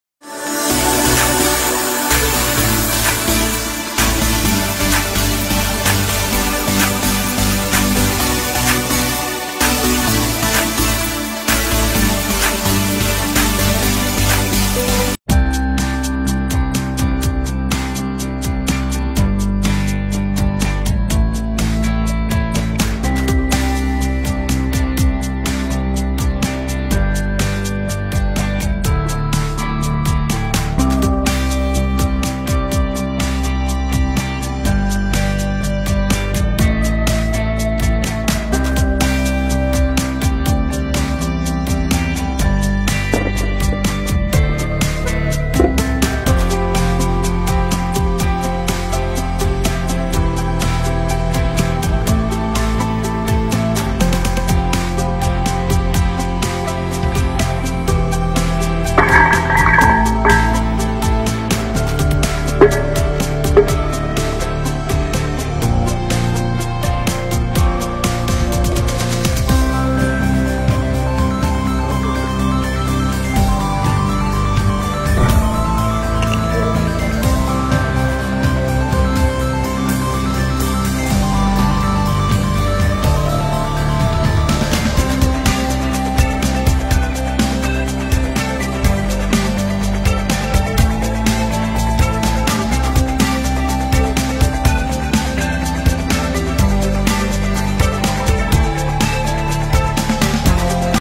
Bottle Cap Challenge: Super Stars Jason Statham, Akshay Kumar, Donnie Yen, Connor Mc Gregor nail it
BottleCapChallenge: Super Stars JasonStatham, AkshayKumar, DonnieYen, ConnorMcGregor nail it.
Can you Do It . The Challenge Is Open . Lets Do It and Send Us The Video